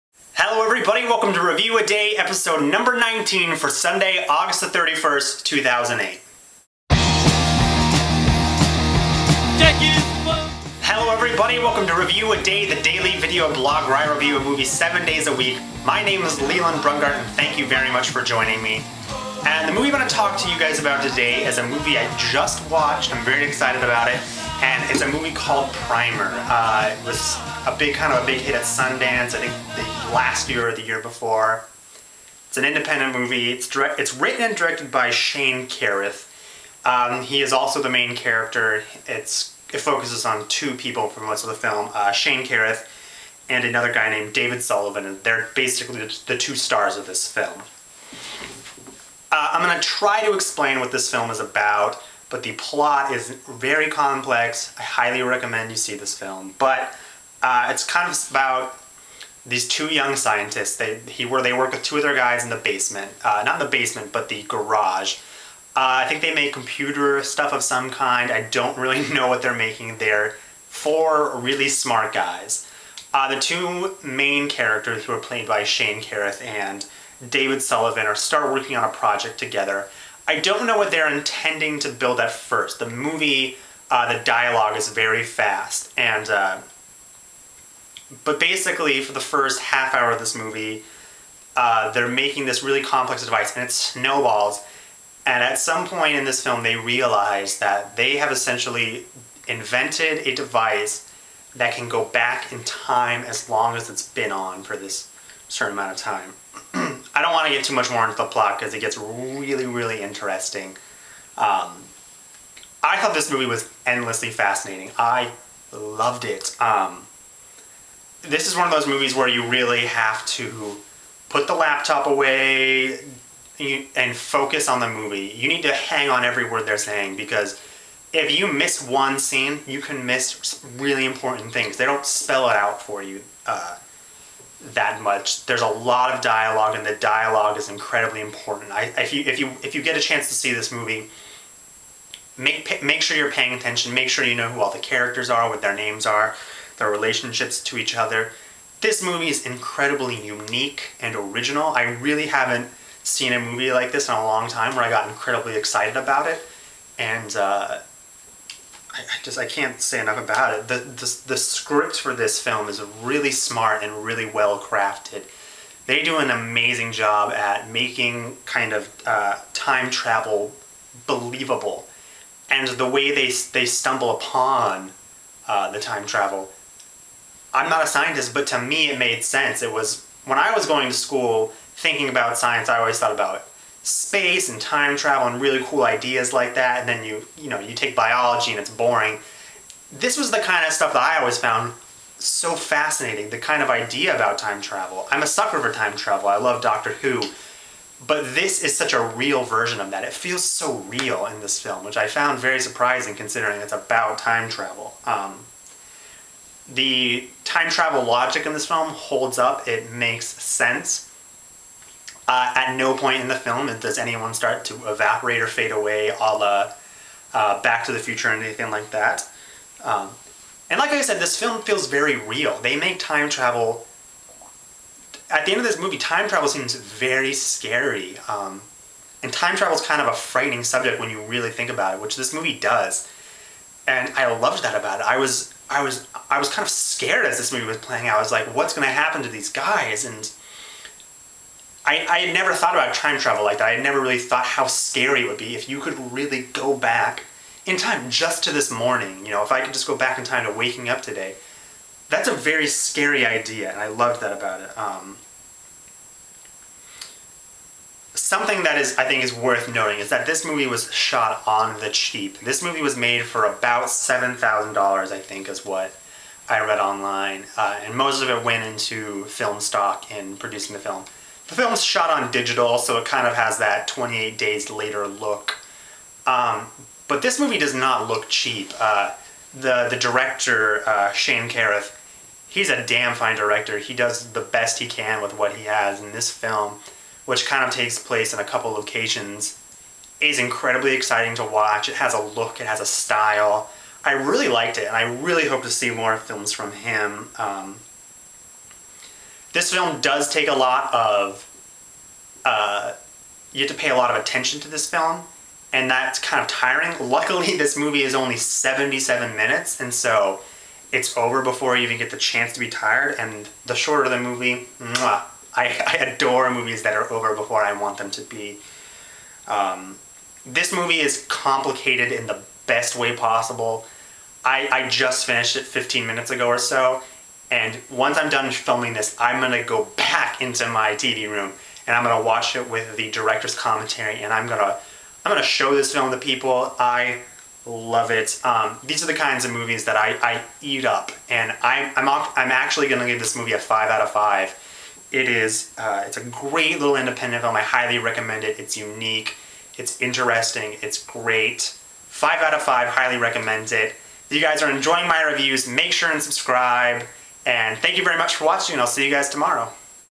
REVIEW-A-DAY #19, Primer
Here's my review of the time bending film PRIMER

My final rating: 5/5

Make sure to tune in 7 days a week for a new movie review!!!

I'll be posting reviews of new releases every Saturday.